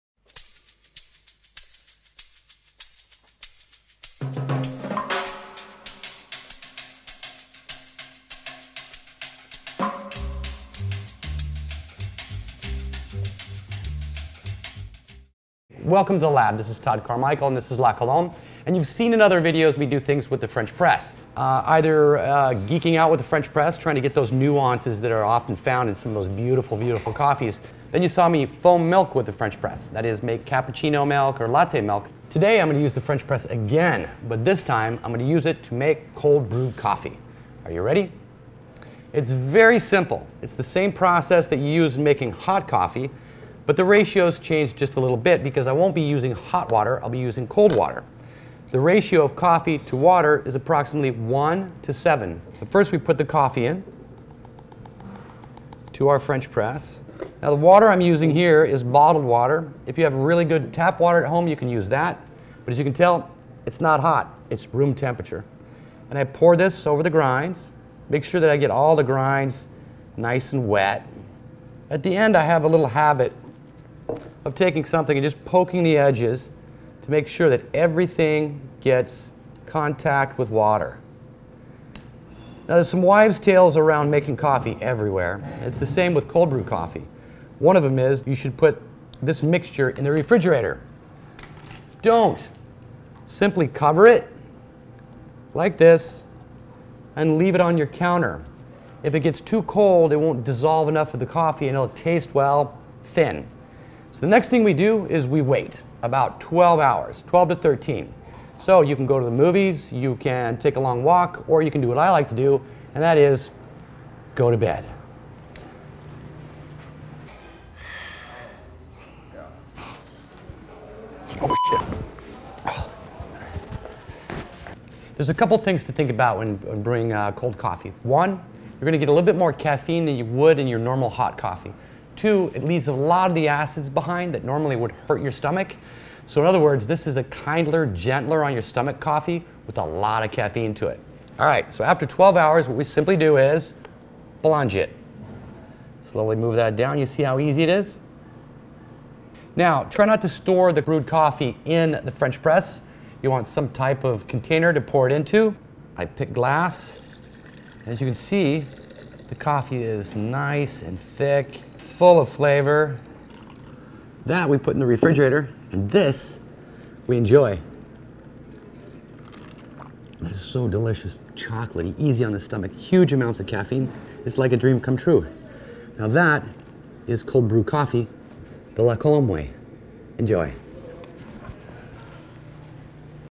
How To: Make Cold Brew at Home
Todd Carmichael explains how easy it is to make cold-brewed coffee using the french press.